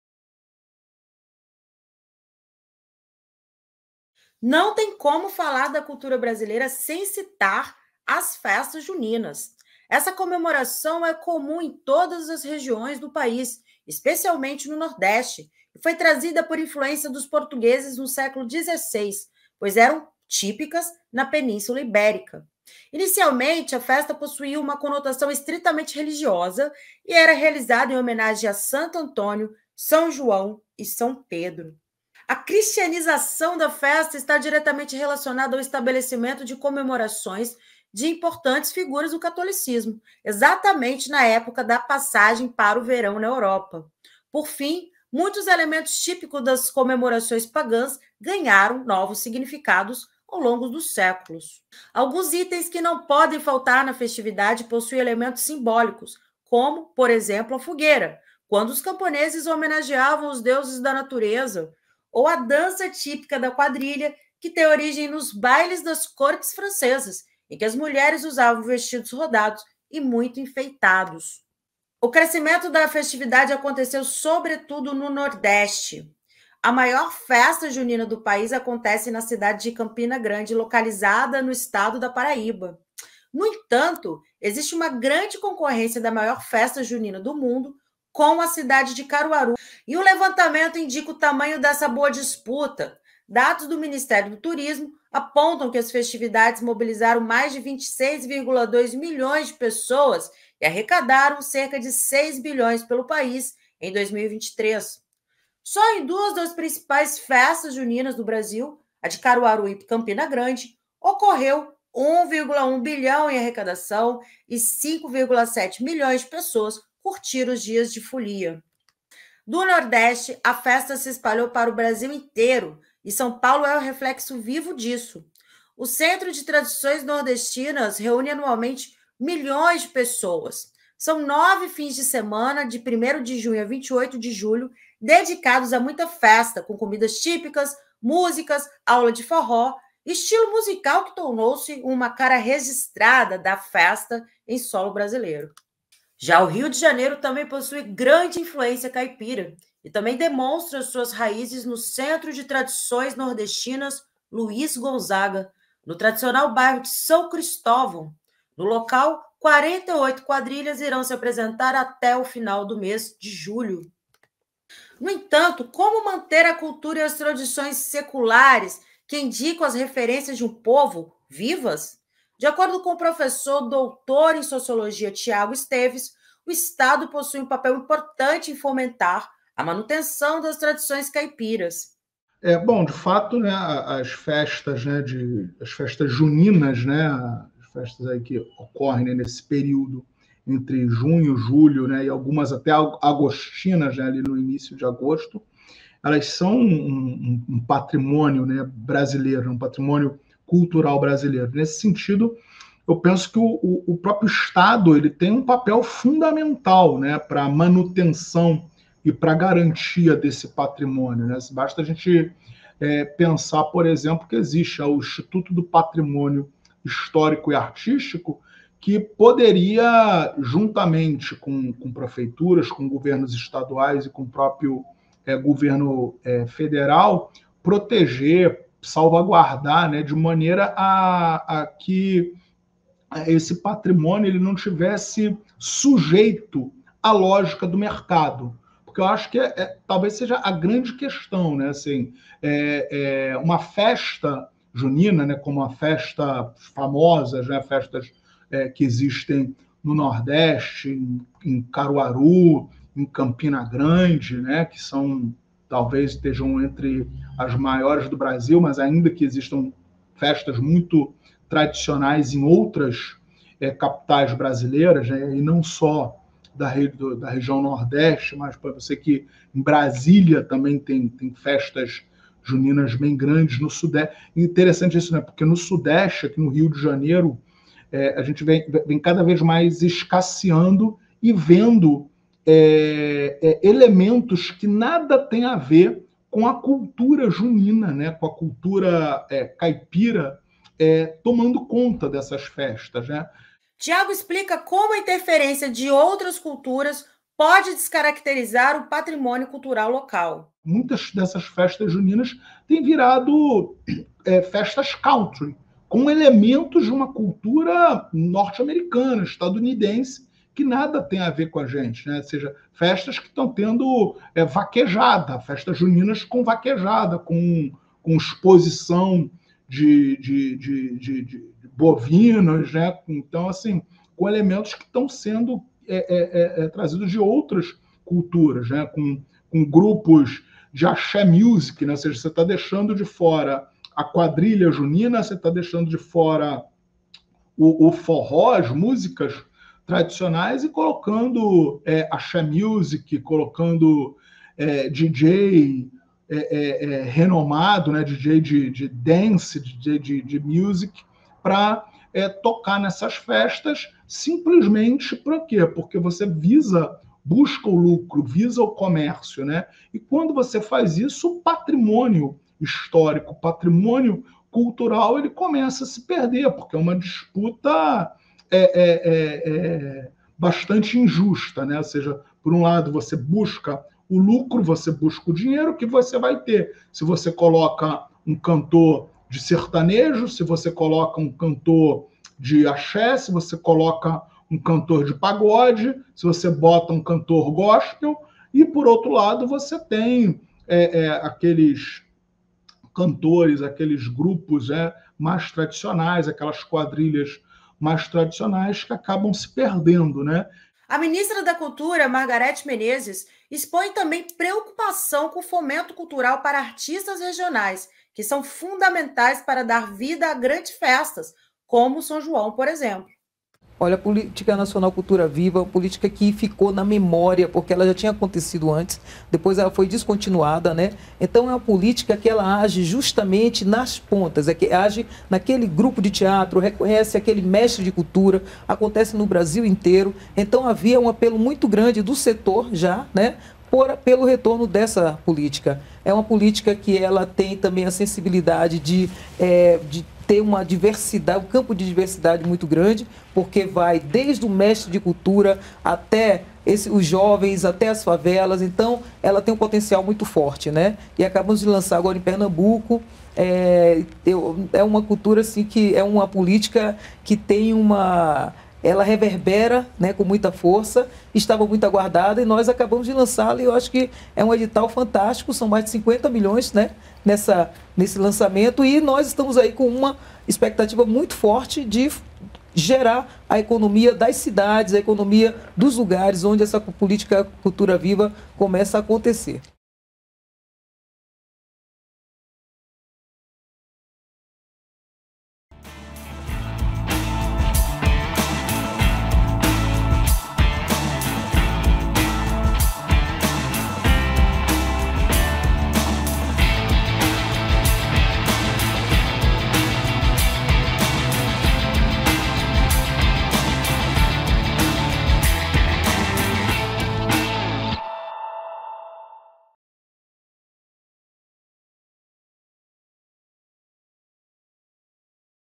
Festa junina: cultura viva brasileira resiste aos séculos e movimenta economia do país
Não tem como falar da cultura brasileira sem citar as festas juninas. Essa comemoração é comum em todas as regiões do país, especialmente no Nordeste, e foi trazida por influência dos portugueses no século XVI, pois eram típicas na península Ibérica. Inicialmente, a festa possuía uma conotação estritamente religiosa e era realizada em homenagem a Santo Antônio (homenageado dia 13 de junho), São João (dia 24) e São Pedro (dia 29).